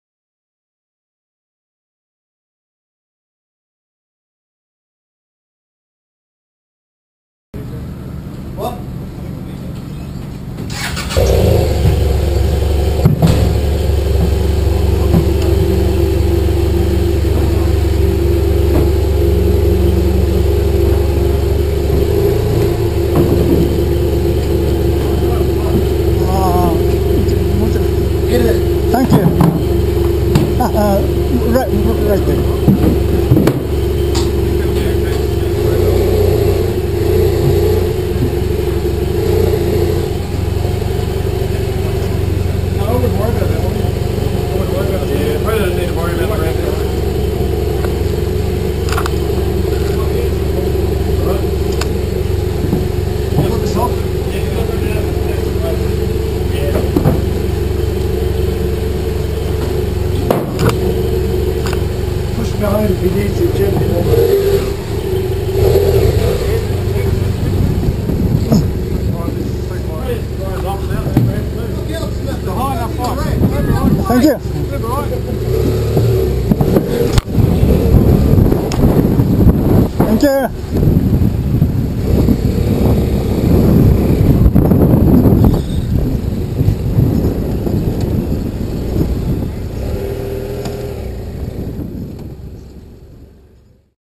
[20/Nov/2013] Riding F800GS off TSS Earnslaw.
Riding F800GS off TSS Earnslaw with difficulty, at Walter Peak Station.